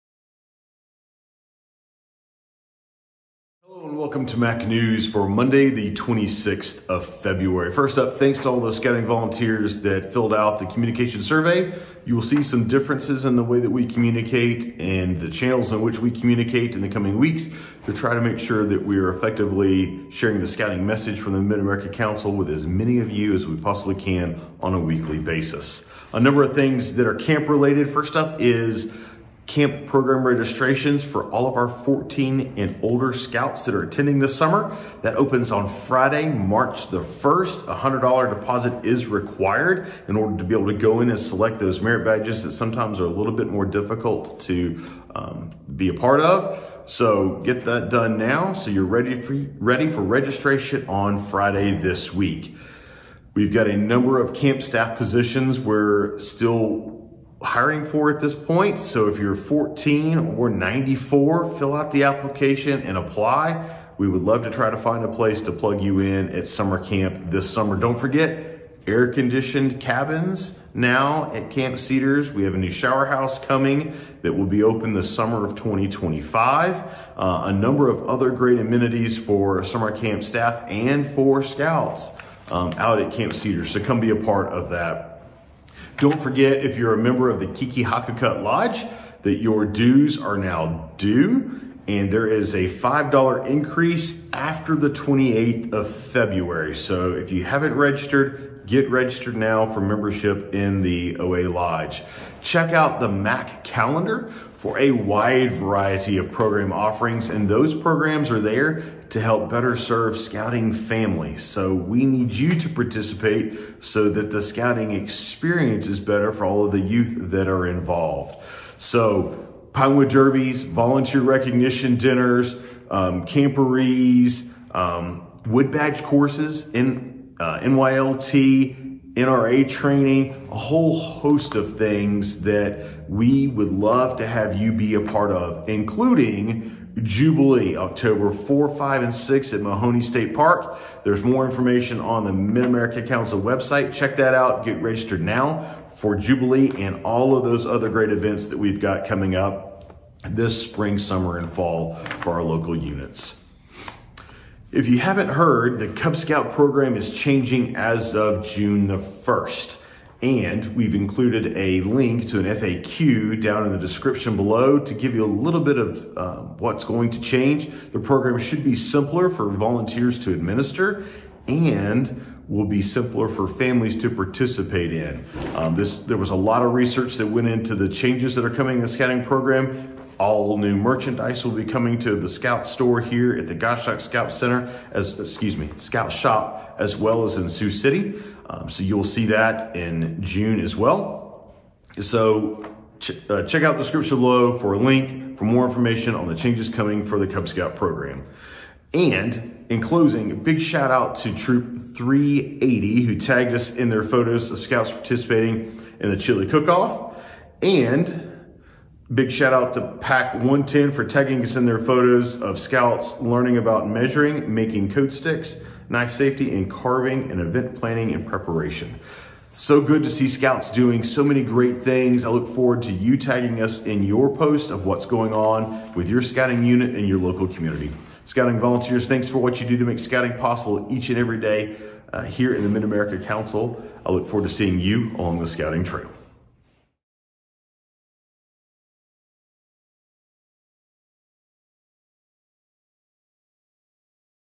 MAC News- 2/26
Program selection for Camp Cedars 2024 opens on March 1 for Scouts 14+ who have made the $100 deposit. Your unit must confirm your participant counts and add in participant details in order to make the deposit and select classes. Parents/ leaders can make a deposit for specific youth at this

OA Dues are increase $5 per person on February 28th. and save $5 per person.

Camp Staff applications are still open and we are looking to fill a few more positions to round out a great camp staff team.

Don’t forget to check out the MAC Calendar to stay up to date on all the future events coming up. You can find the link to the calendar below.

FACEBOOK SHOUT OUT
Shoutout to Troop 380 for tagging us in their photos of Scouts participating in a Chili Feed.
Shoutout to Pack 110 for tagging us in their photos of their scouts learning about measuring & making code sticks, knife safety & carving and event planning/preparation.